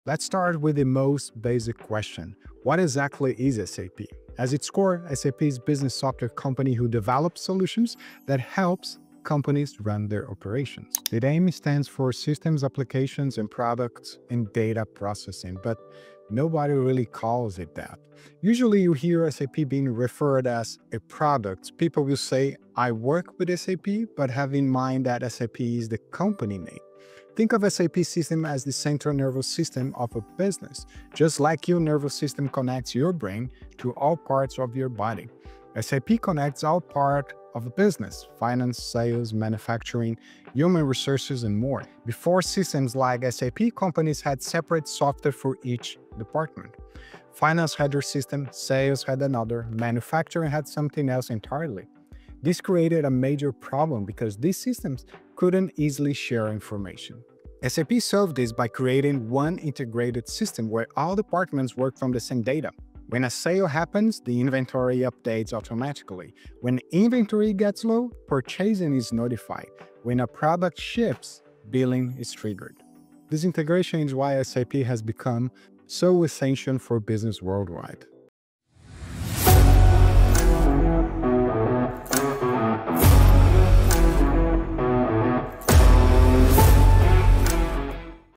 SAP Explained So Even Kids Can Get It!
Let's understand "what is SAP" in simple terms. As a business software company, SAP helps businesses manage their "business operation" efficiently. The system acts like a "data processor", connecting various business processes for "efficiency optimization".